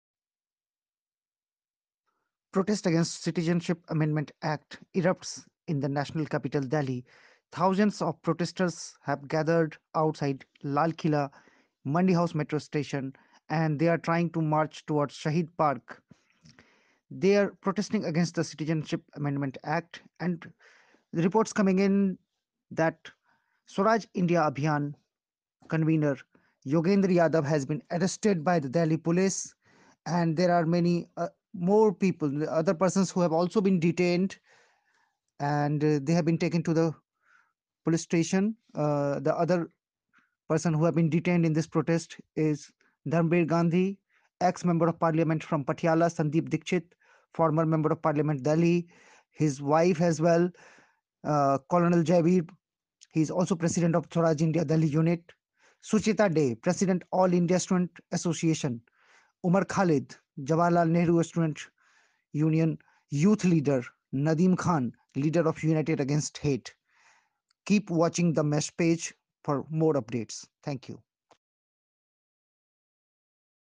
CAA Protest Delhi: Police detained leaders including Yogendra Yadav and Sandeep Dixit at Mandi House
CAA Protest Delhi: Police have detained several people and their leaders while protesting against CAA at Mandi House. Here are some names in the List of detained people- Dharamvir Gandhi, Ex-MP Patiala, Sandeep Dixit, Ex-MP, Delhi, Yogendra Yadav, President Swaraj India,Col. Jaiveer, President Swaraj India Delhi unit,Sucheta De, President AISA,Umar Khalid, Youth Leader
Nadeem Khan, Leader United Against Hate. Reports coming in that protesters continue to march towards Shaheed park.